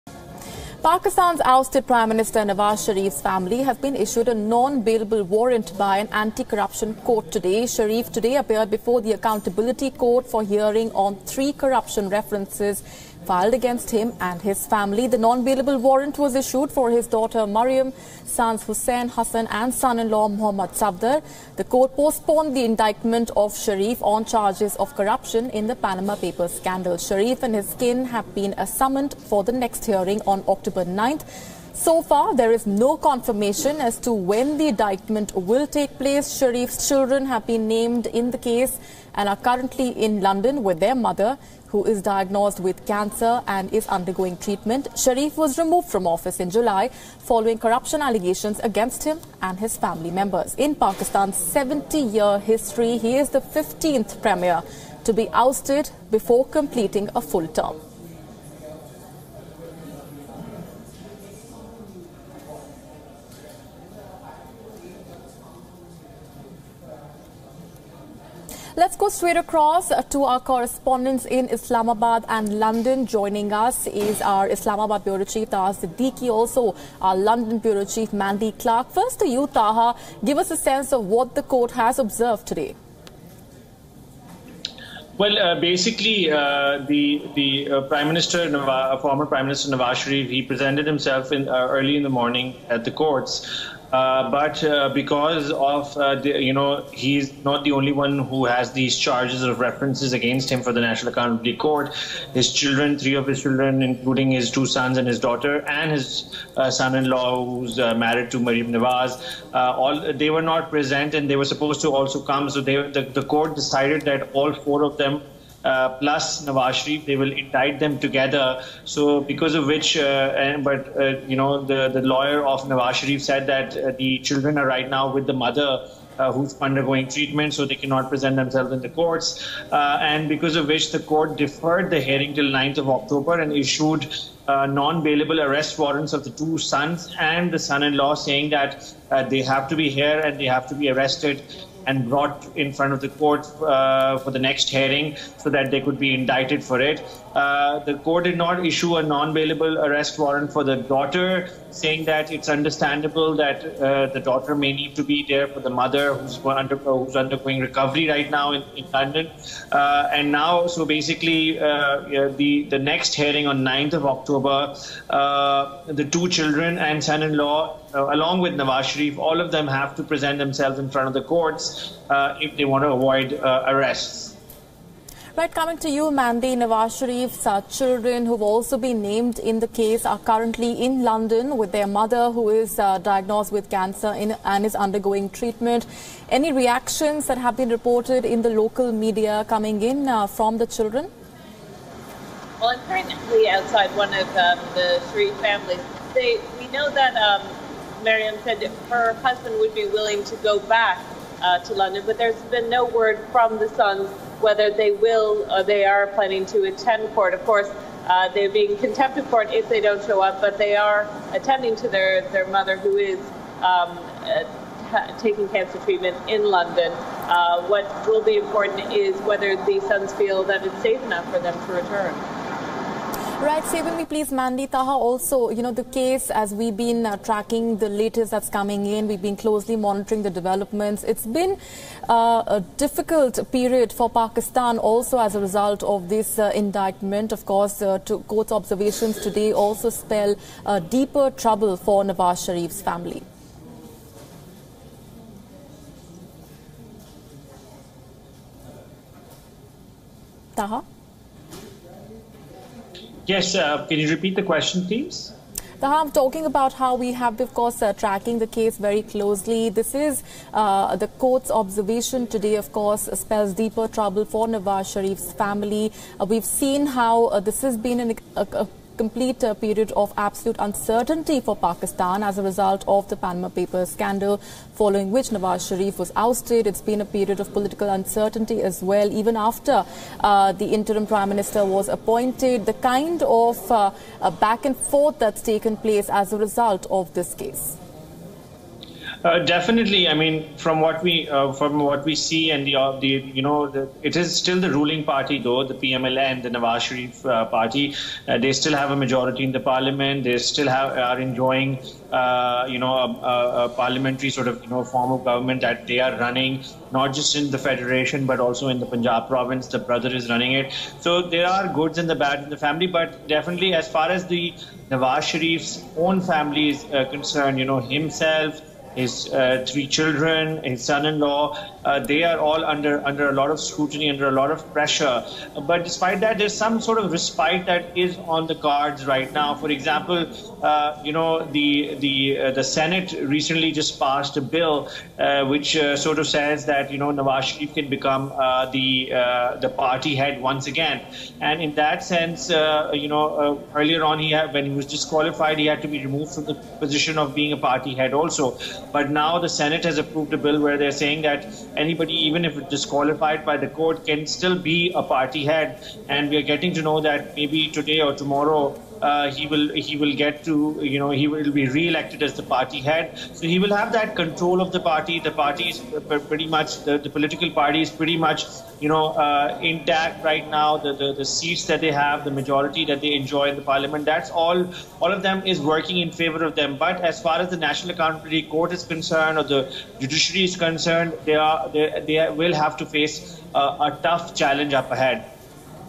Nawaz's kin issued non-bailable warrant by an accountability court
This segment of WION brings you latest updates on non-bailable warrant issued against Nawaz's kin by an accountability court. watch this clip to know more.